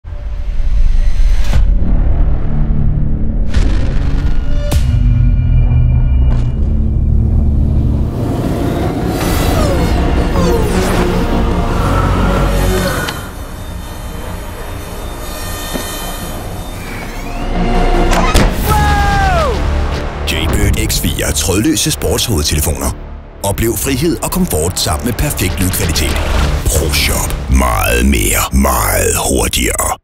Proshop – meget mere meget hurtigere - Jaybird - 30 sekunder
Jaybird X4 trådløse sports hovedtelefoner - Oplev frihed og komfort sammen med perfekt lydkvalitet.